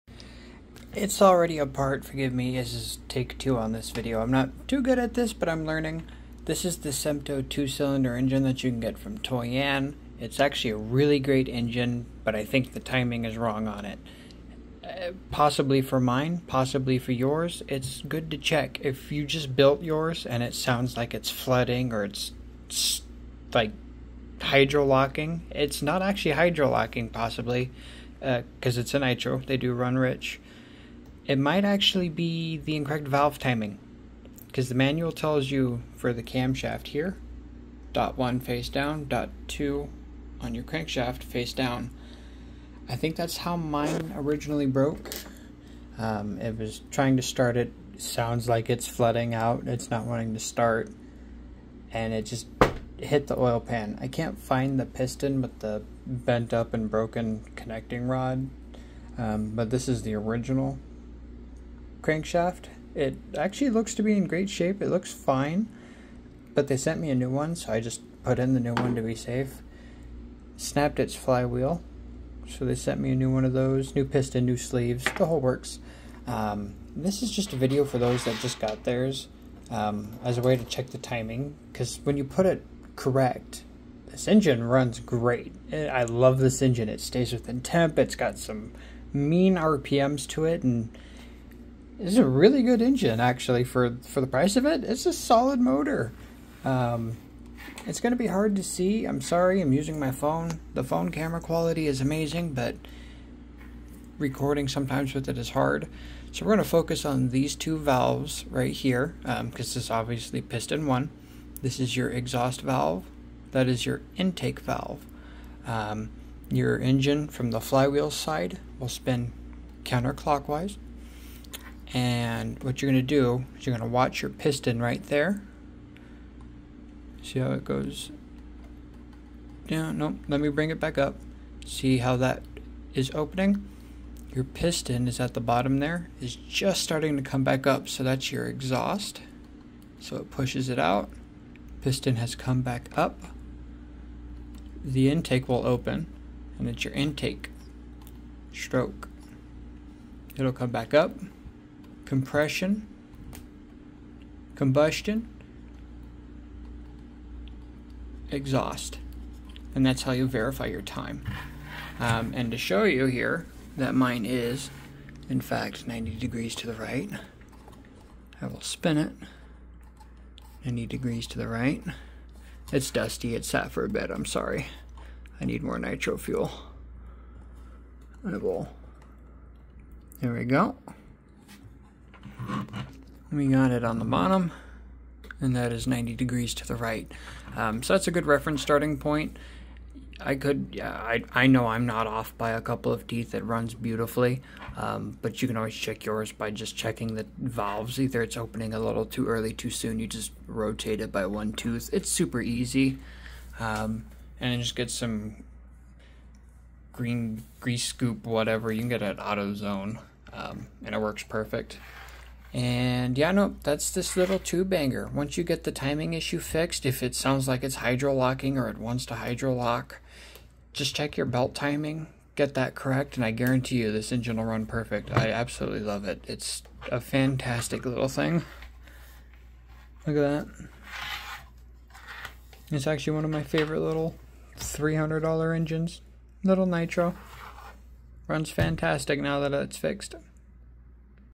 How I (might) have resolved the horrible reliability on the Toyan FS-L200
The manual states both dots face down, I don't think that's correct. And possibly why a lot of these engines fail. Or refuse to start. This video shows how I discovered/ fixed that issue on my engine.

My engine now runs beautifully, has strong rpms, and has awesome temps. I have full confidence in my engine now! And has run 3 tanks with 0 issues on starting. Running and / or idling without glow warmers staying on the plugs!

I say that's pretty good!